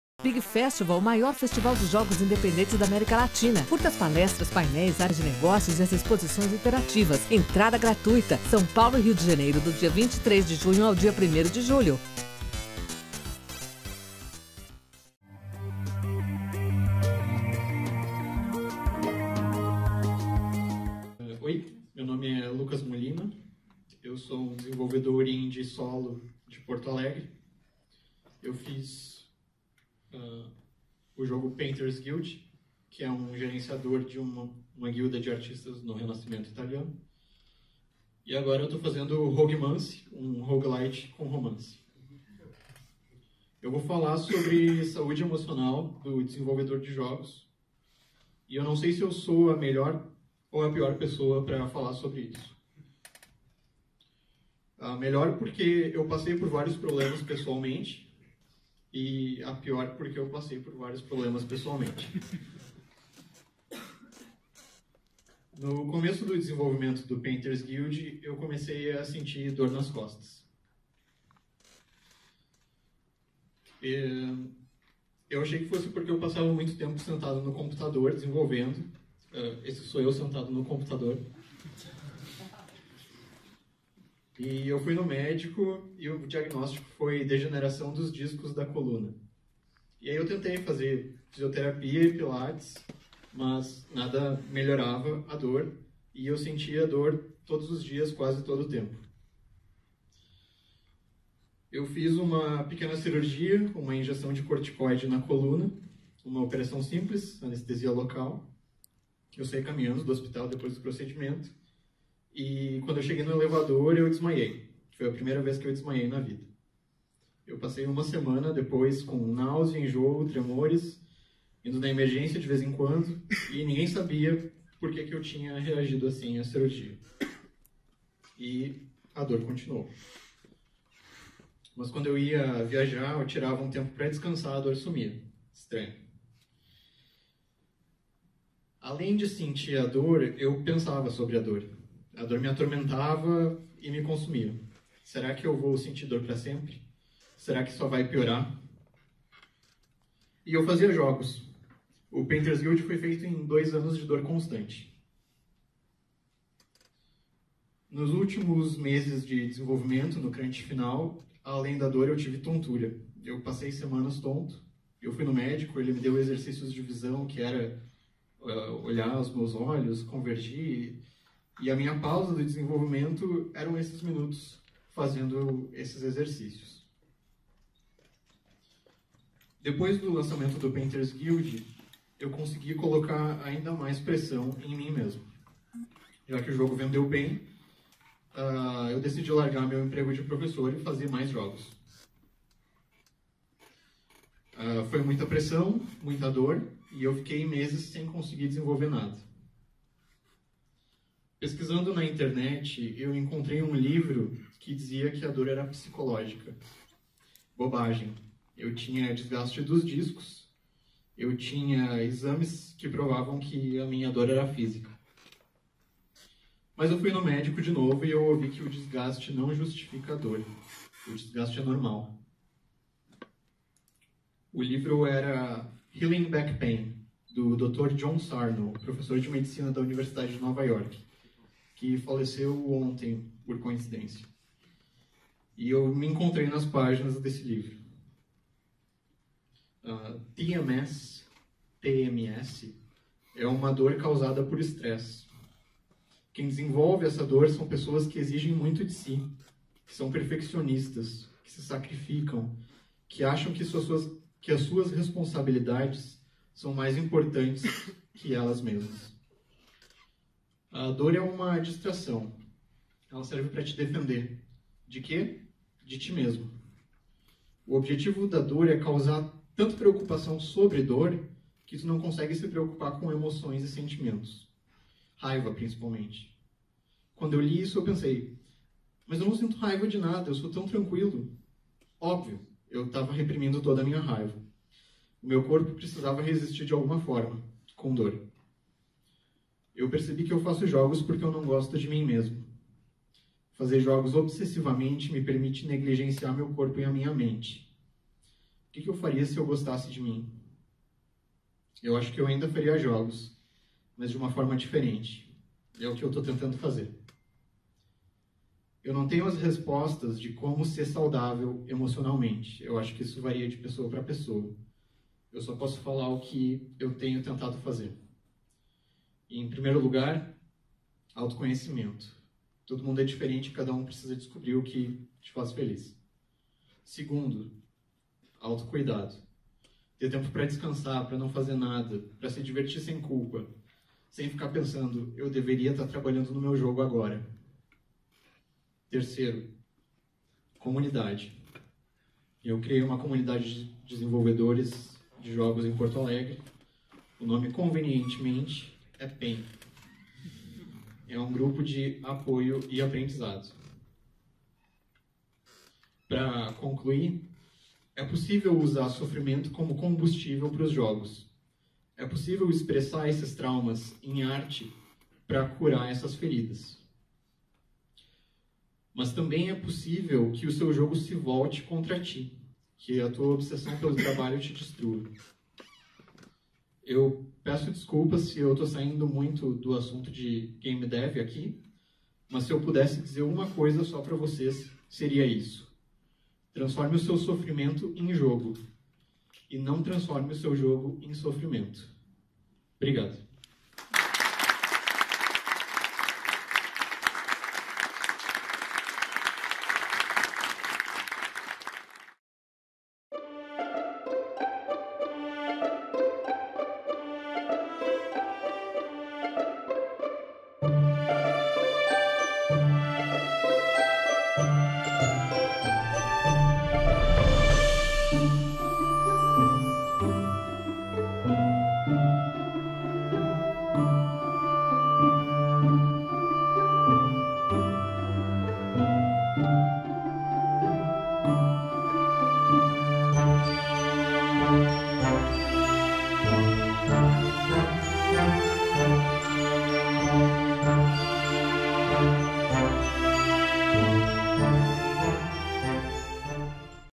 Lucas Molina - Saúde emocional do Desenvolvedor de Jogos - Soapbox - BIG Festival 2017
Acompanhando o sucesso da edição passada, o BIG Festival recebe o segundo round do Soapbox, a série de microtalks sobre diferentes perspectivas do desenvolvimento de jogos. O objetivo é sair da caixa e alcançar novas ideias e pontos de vista distintos.

Palestrantes: Leonardo Ferreira (In Extremis), Gustavo Foletto (IMGNATION Studios), Thais Weiller (JoyMasher), Lucas Molina (Roguemance), Rafaella Ryon (Ninja Garage), Betu Souza (Behold Studios)

Moderador: Saulo Camarotti (Behold Studios / Abragames)